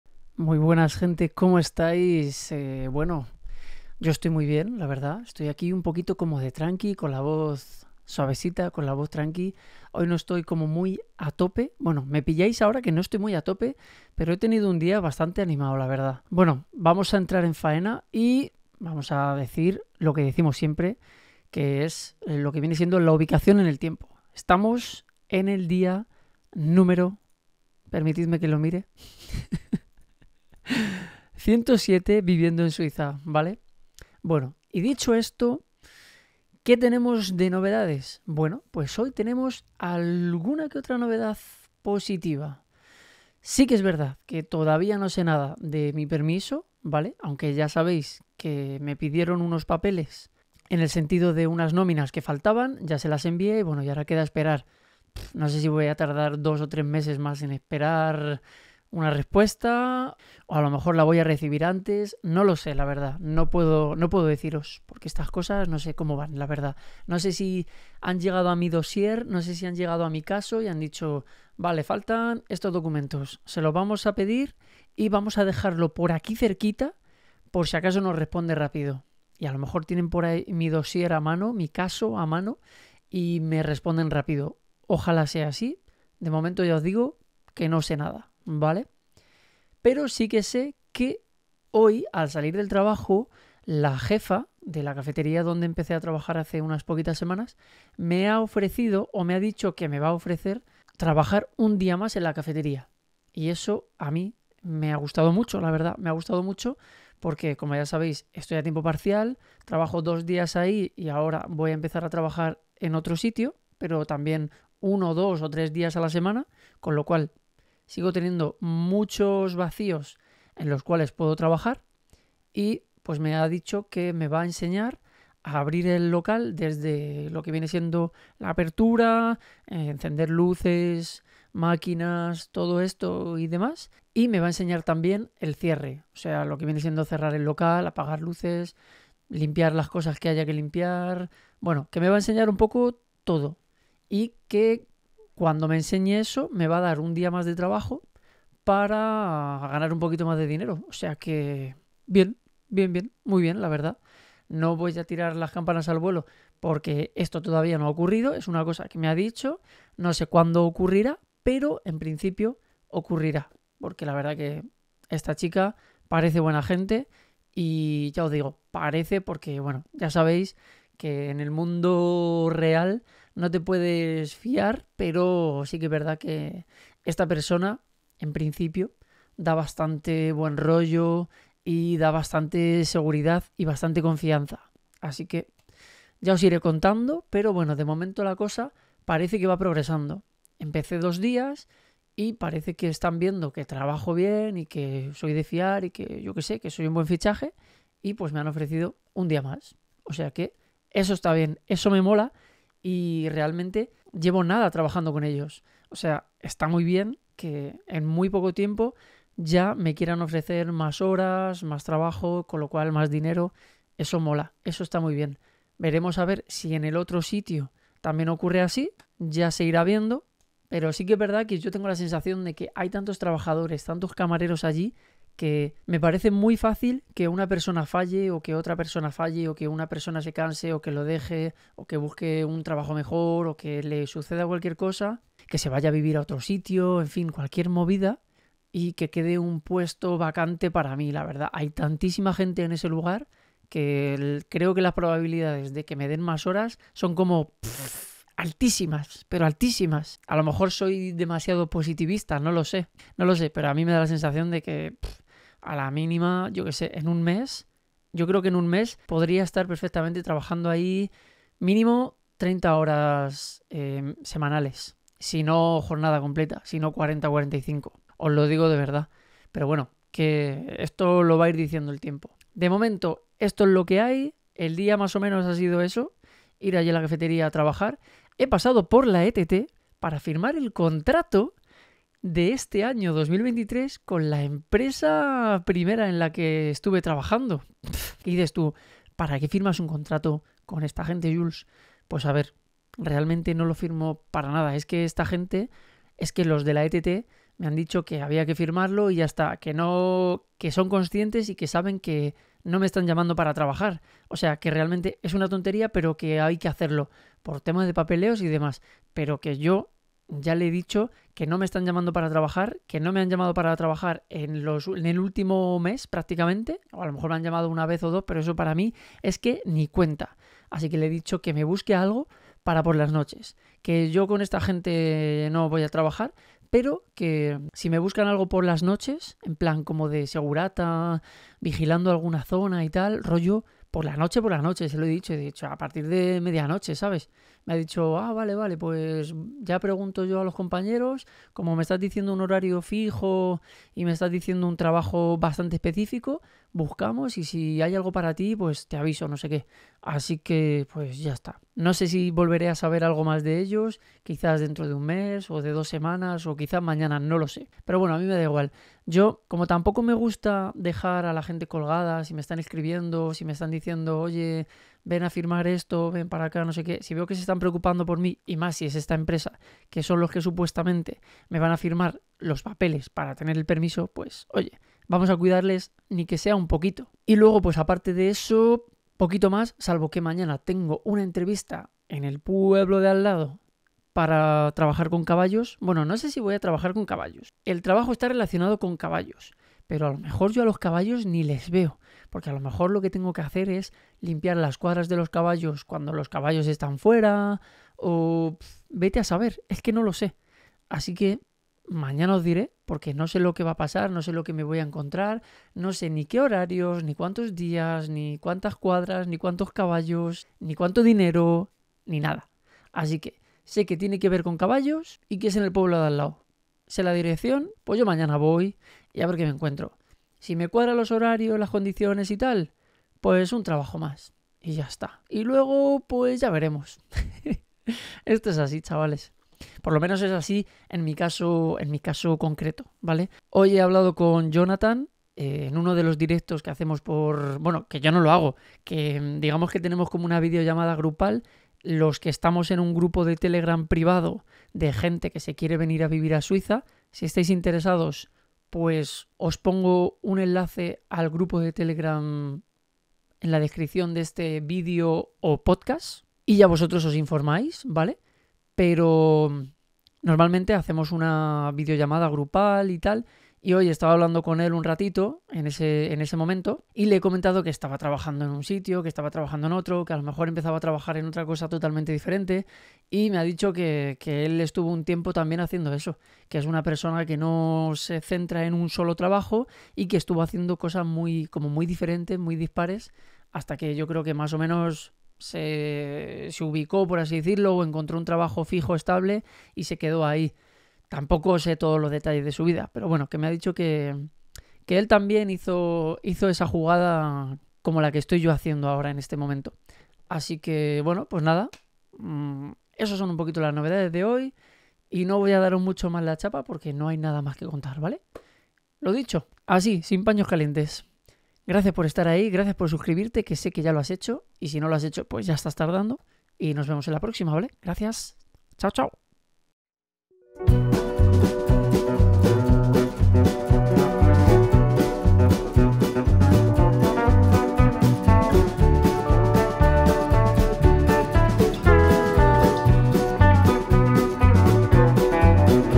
Viviendo en Suiza - Día 107, el de ser polivalente
Ser polivalente tiene sus pros y sus contras. En mi caso, lo veo más una ventaja que me permite poder acceder a diferentes tipos de trabajo. Obviamente, son trabajos no excesivamente cualificados. En este capitulo os cuento cómo va mi actualidad laboral y alguna cosita más. Venga, prepara palomitas y dale al play. Por cierto:

Ya está disponible mi videovlog en version podcast en Spotify y Amazon Music: Vivir en Suiza con Jules O'Connor